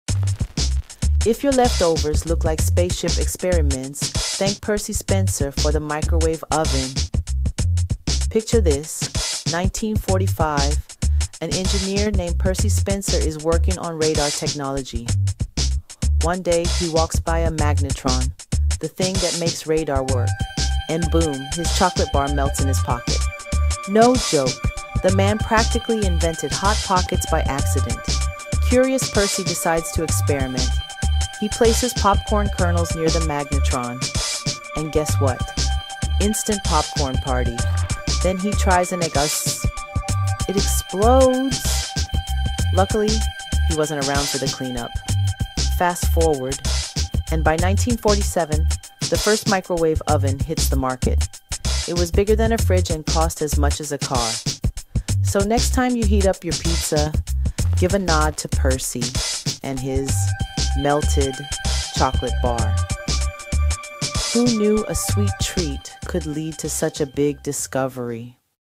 The Snack That Invented the Microwave📻🍫microwave
The microwave oven was discovered completely by accident in 1945 by engineer Percy Spencer. While working on radar technology, he noticed a candy bar in his pocket had mysteriously melted. Intrigued, he experimented with popcorn and even an egg—which exploded! His curiosity led to the invention of the first microwave oven, transforming kitchens forever. From radar waves to popcorn in seconds, this snack-fueled mistake changed the way we cook.